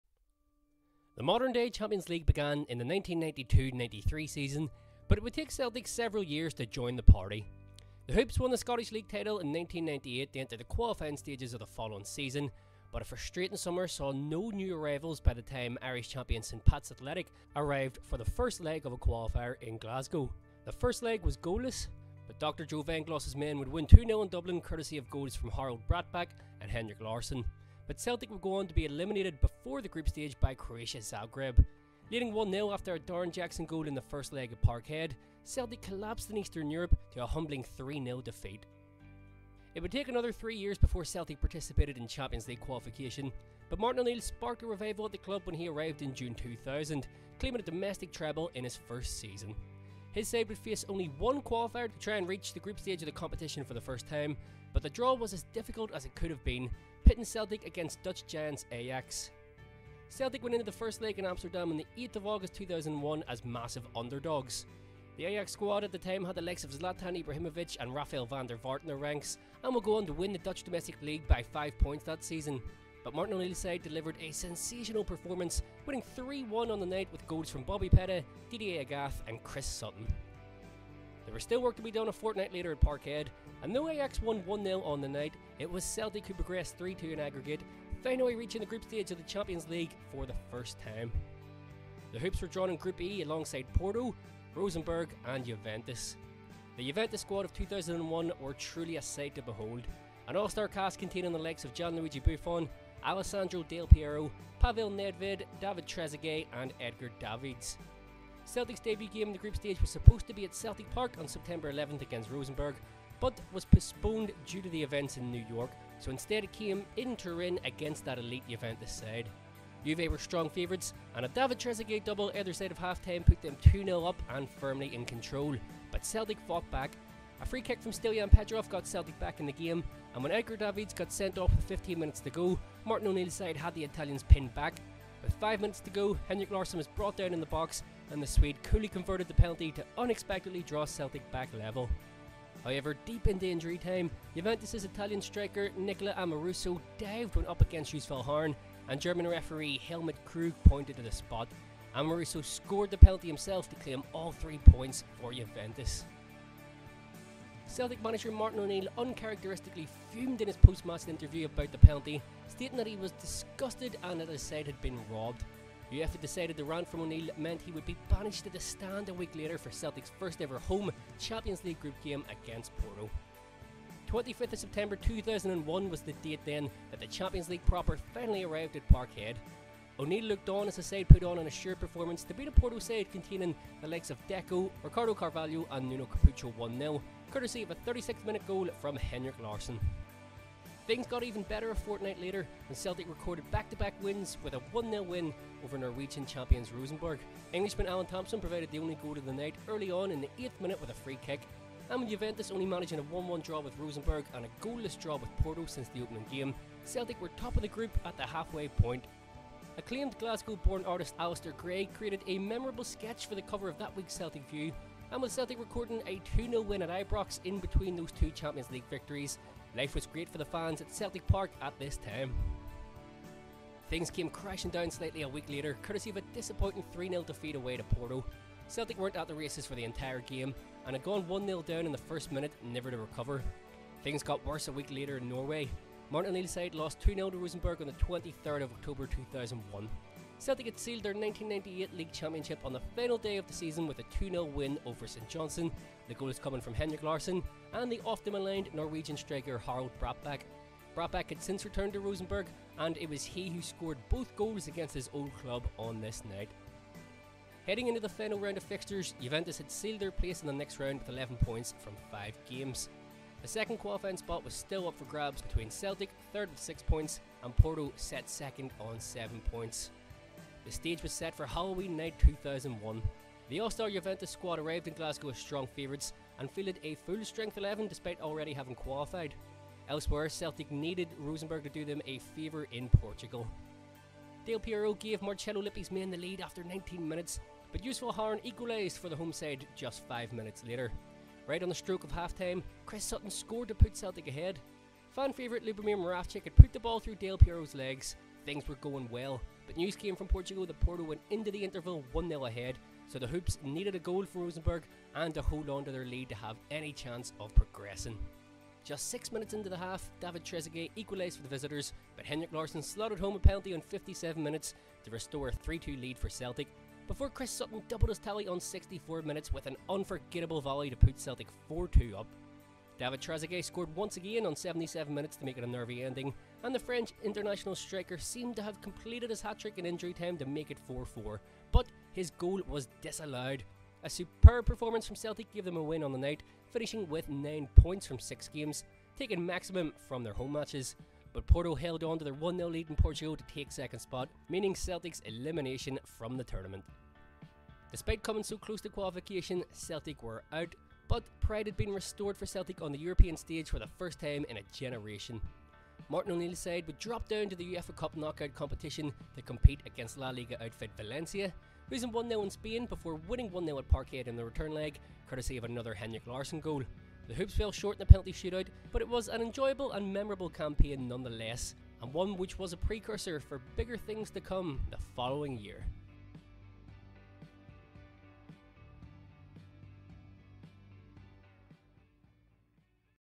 Celtic Stories- Celtic's First Champions League Group
The story of Celtic's first campaign in the Champions League proper, season 2001/02. Martin O'Neill's side had to overcome Ajax in qualifying to be drawn in a group alongside Juventus, Porto and Rosenborg.